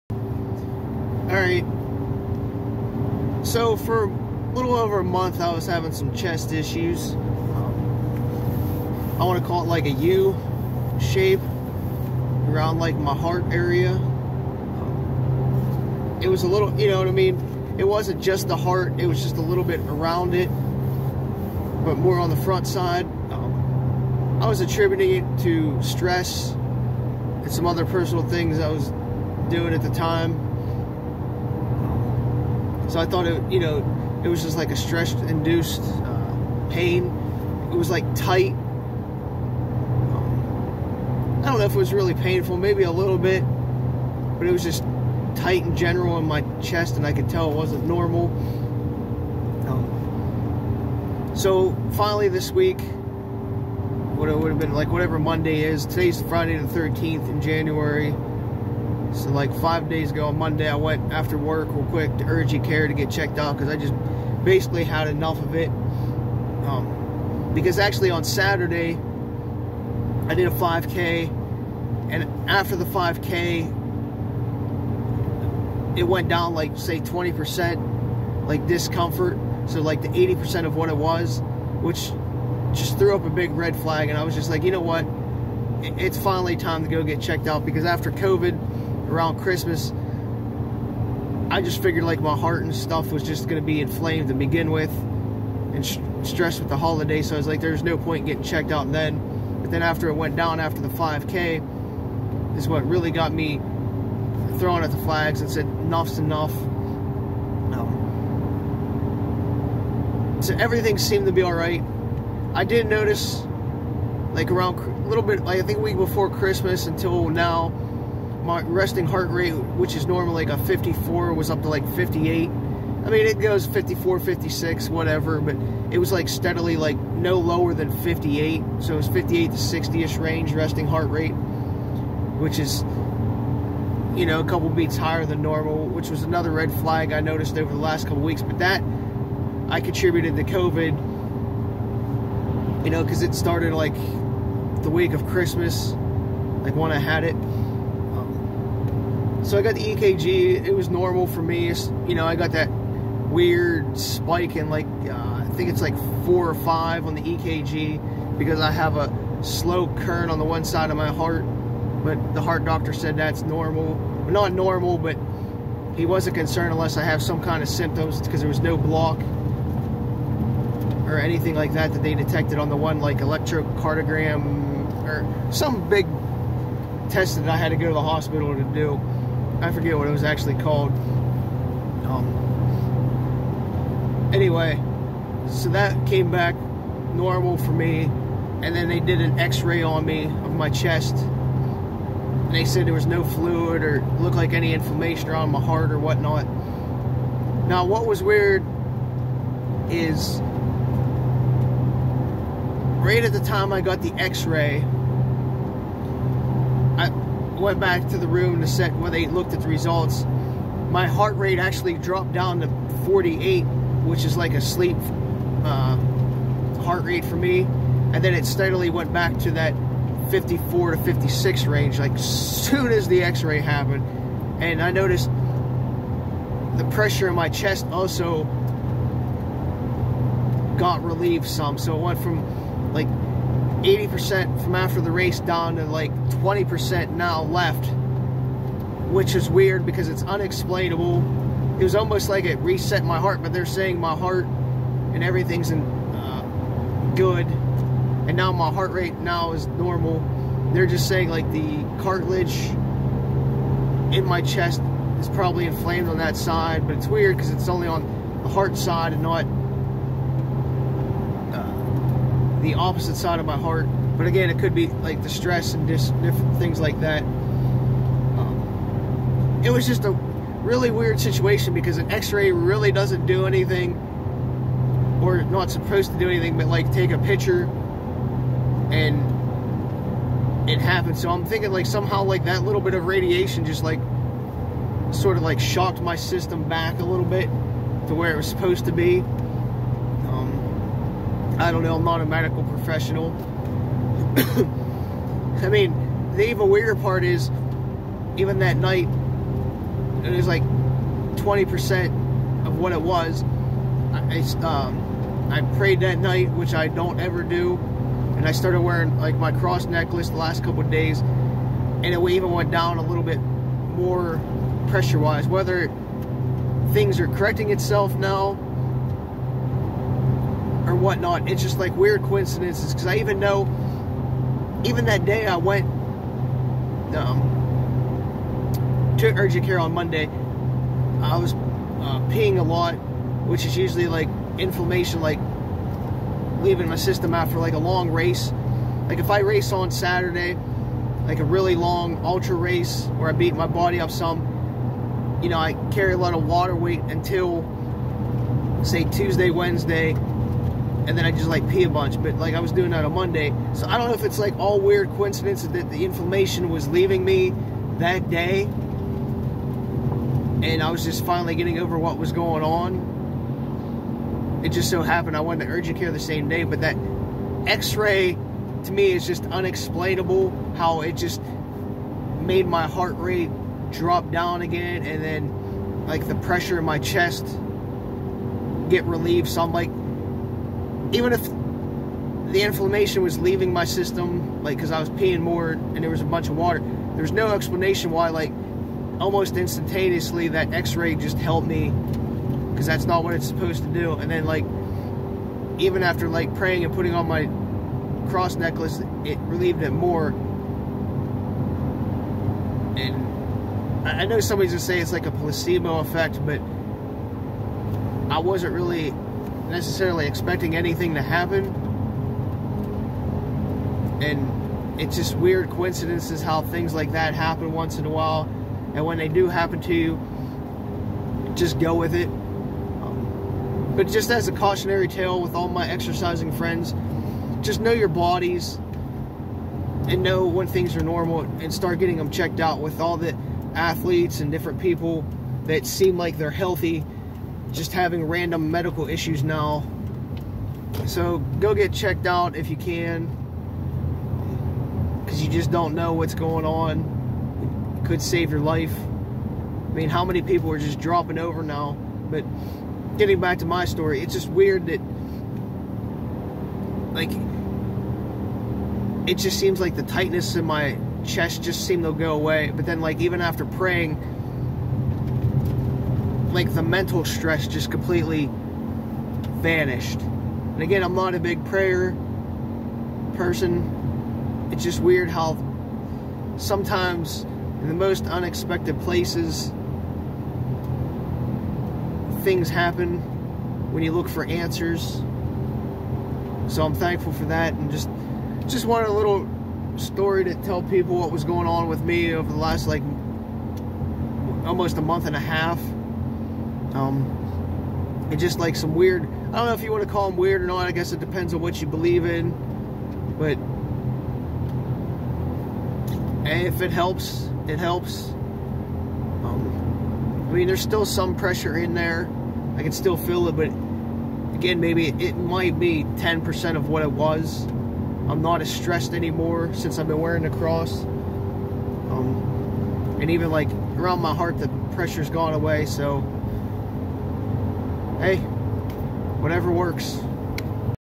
Heart Problems I was Having
Was having some heart/chest problems. This explain what was going on amd what weird things that fixed it that can't be explained. Athletes should be cautious.